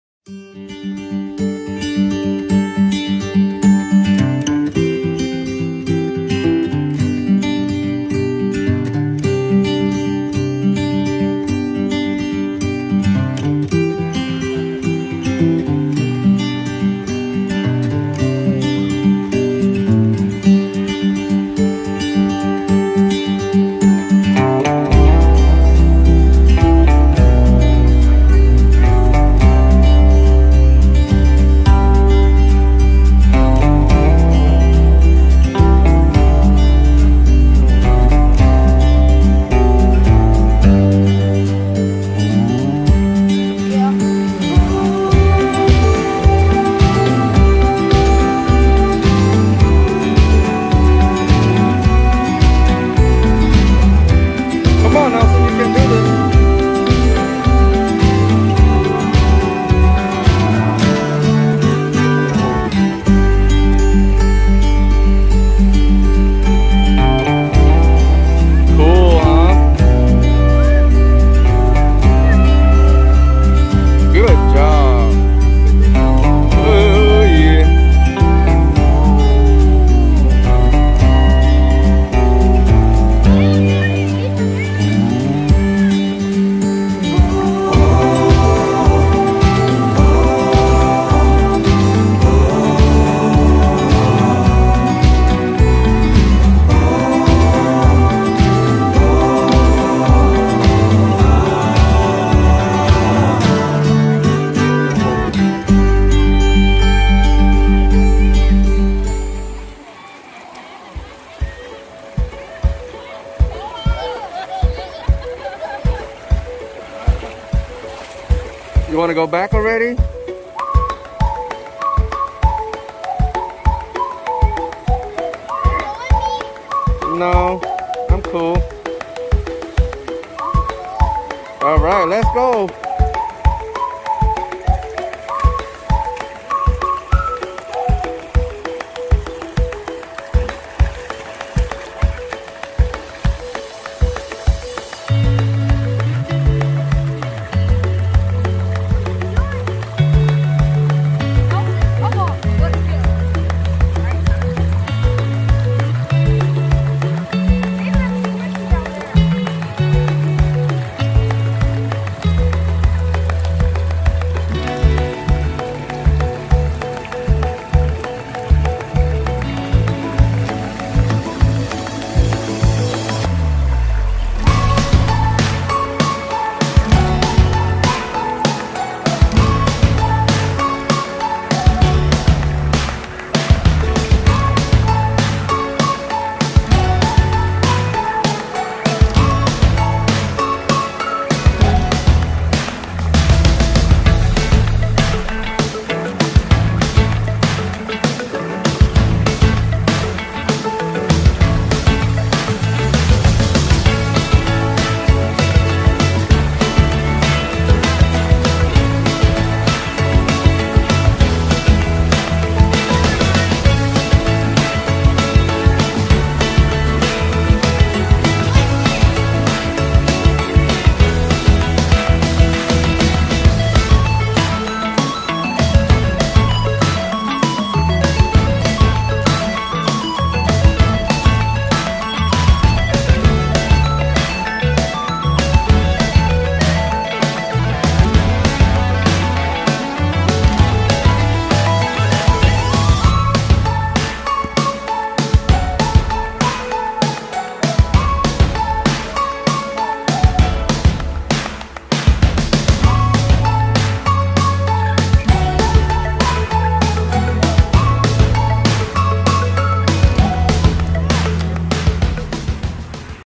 First Weekend of Spring 2021 at Clay Banks - William B. Pond
Nathan, Nicholas, and Nelson having a good time at Clay Banks, William B. Pond.